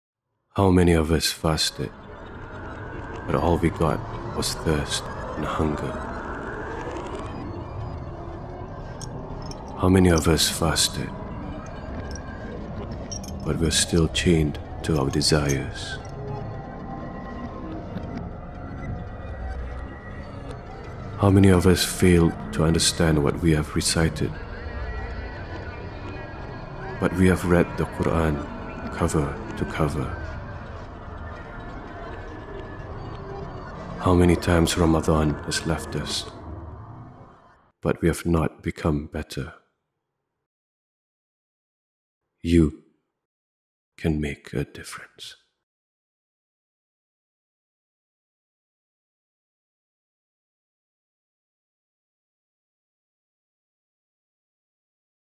How Many of Us Fasted?
A short film from a short poem about fasting in the month of Ramadhan. May this Ramadhan be the best one.

Directed by Abu Yahya At-Tamantunni
Co-directed by Deeni

How many of us fasted, but all we got was thirst and hunger?
How many of us fasted, but we are still chained to our desires?
How many of us failed to understand what we have recited, but we have read the Qur'an cover to cover?
How many times Ramadhan has left us, but we have not become better?